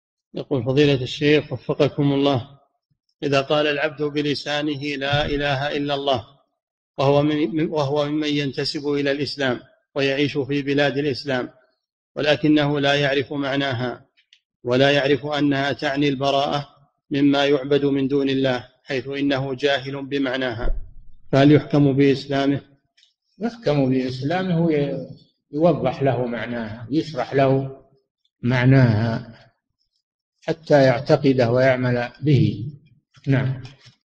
[30 -521] من قال "لا إله إلا الله" ولا يعرف أنها تعني البراءة مما يعبد من دون الله فهل يحكم بإسلامه؟
-[30 -521] السؤال: إذا قال العبد بلسانه: لا إله إلا الله، وهو ممن ينتسب إلى الإسلام، ويعيش في بلاد الإسلام، ولكنه لا يعرف معناها، ولا يعرف أنها تعني البراءة مما يُعبد من دون الله، حيثُ إنه جاهلٌ بمعناها، فهل يُحكم بإسلامه؟
الجواب: يُحكم بإسلامه ويوضح له معناها يُشرح له معناها حتى يعتقده ويعمل به.
للاشتراك في قناة الشيخ صالح الفوزان

للاستفسار والإقتراحات والدعم
رئيس الفريق
لمشروع كبار العلماء